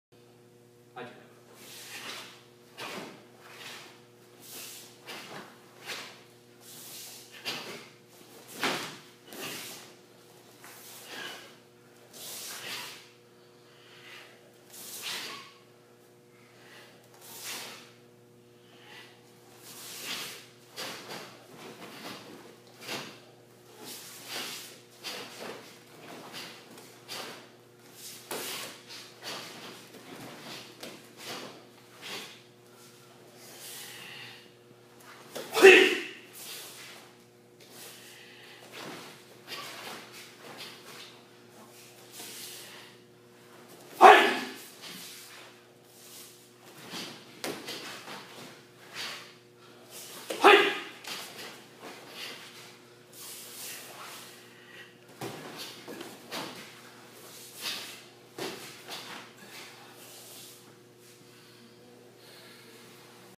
Seisan
Bill Goebel Sensei and his students practice Seisan Kata.